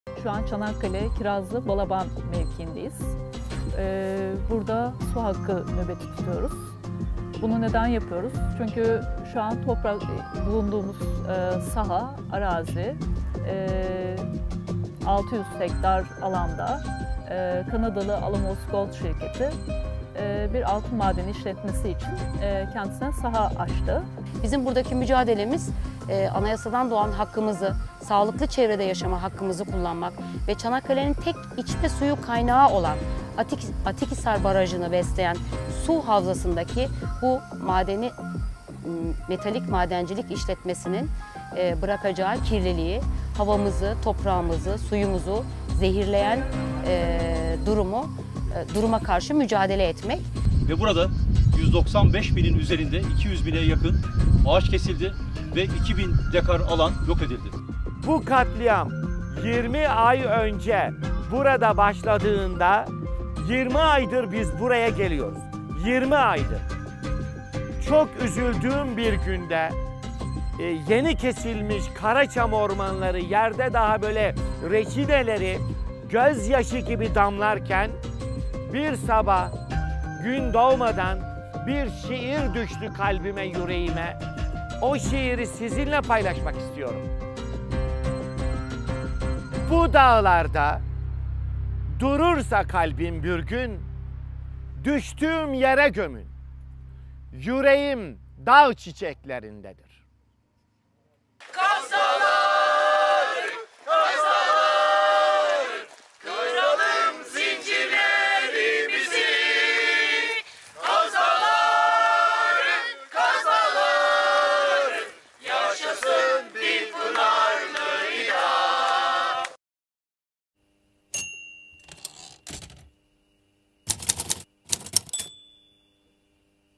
Mt. Ida: “Long live Mt. Ida, the one with the thousand springs”
We recorded the ecological devastation in Mt. Ida where thousands of trees cut off by Canadian firm Alamos Gold because of gold mining.

Video: Volkan Işıl, Onur Temel, Öncül Kırlagıç